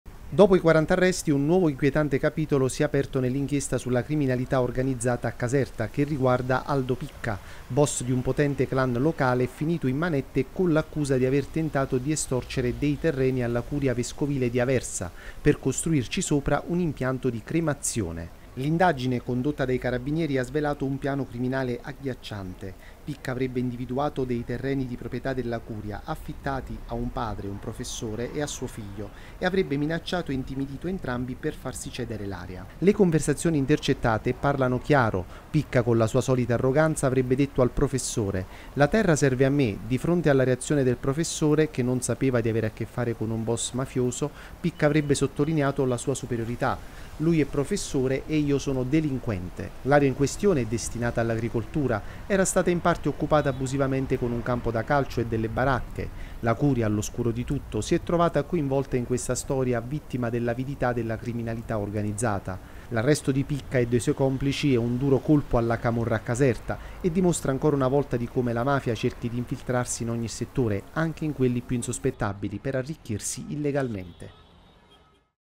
Caserta, il clan Picca voleva costruire un forno crematorio su terreni Curia
Dopo i 40 arresti, emergono dettagli inquietanti sulle attività del clan Picca in provincia di Caserta. Il boss Aldo Picca si sarebbe appropriato con le maniere forti di un terreno di proprietà della Curia su cui voleva costruire un forno crematorio.